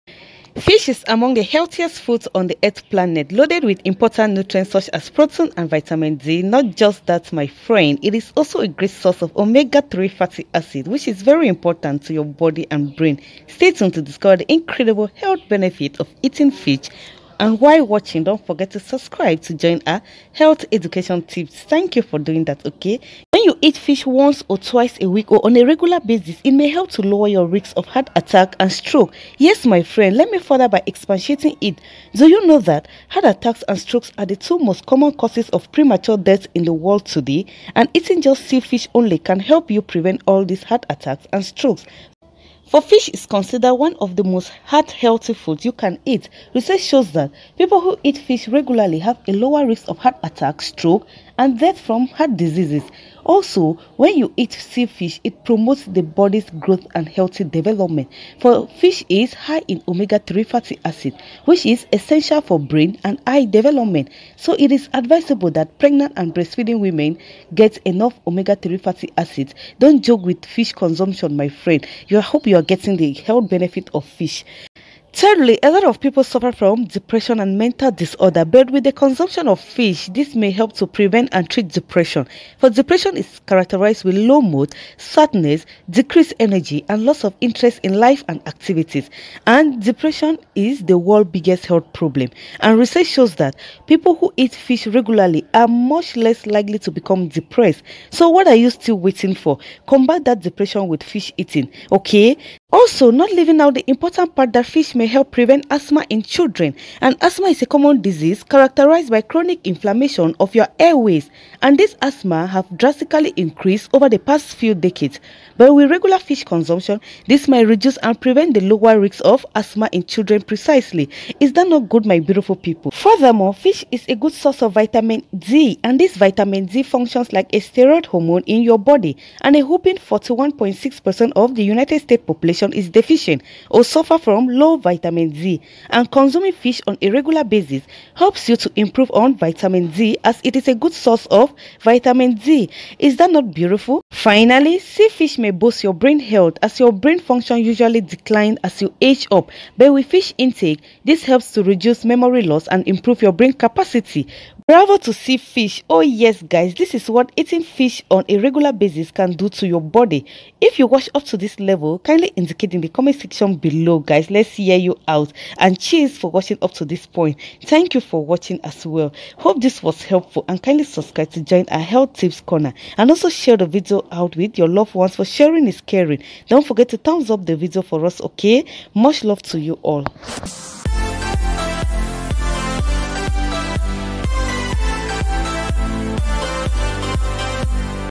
If You Eat Sea Fish Everyday For A Week This Will HappenTo Your Body/Sea Fish Health Benefits
Hello health lovers here is what sea fish does to your body when you consume it on a regular basis ,you will be surprised and amazed of it's health benefits of sea fish.Thank you for watching and please kindly subscribe to join our healthy community cheers